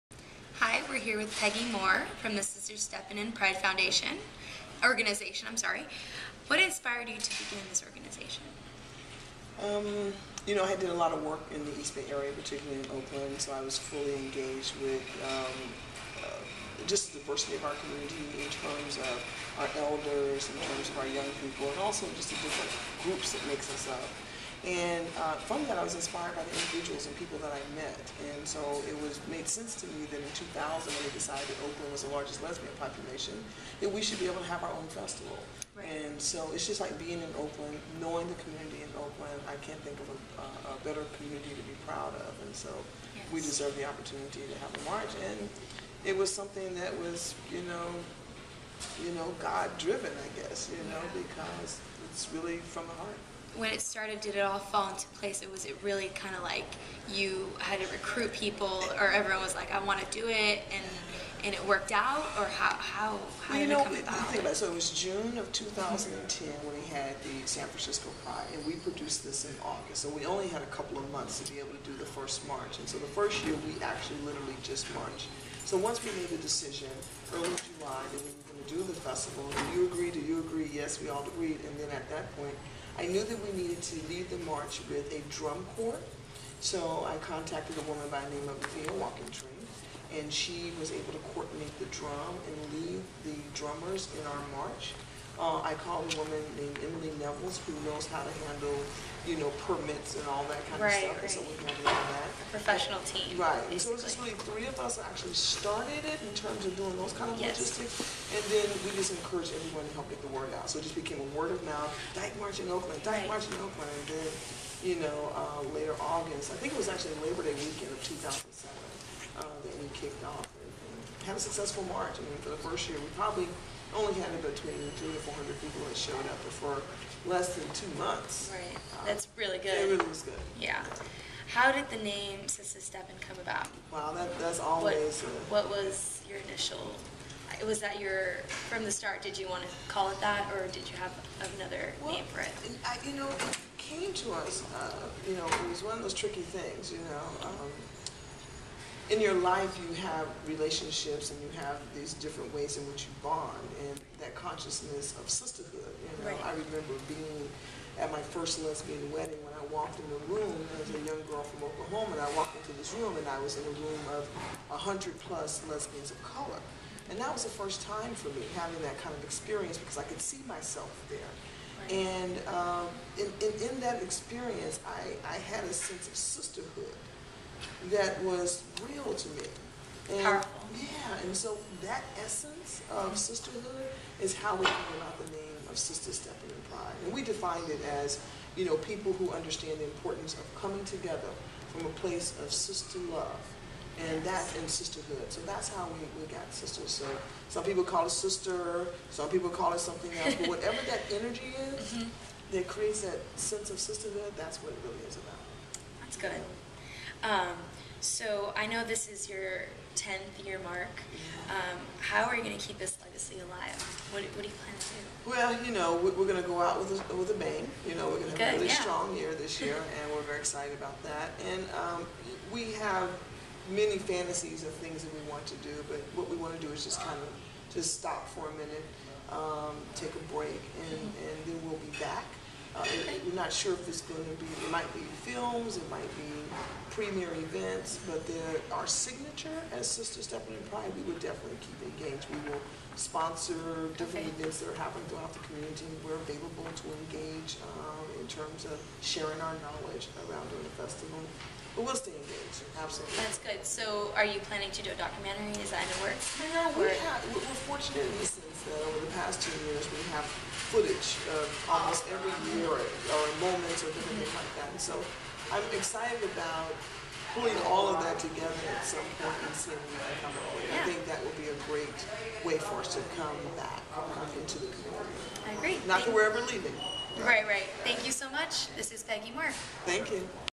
Peggy Moore interview on Sistah's Steppin in Pride
Peggy Moore, Founder of Oakland's Sistah's Steppin in Pride organization.

SSIP is the pride, beauty, and talent of our queer women's community, seen through a fun-loving, family-reunion festival of activities. SSIP comes together to
create a safe space where participants can inspire each other to dream, manifest and love. In fact, 2011 is the tenth (and last) annual march/festival.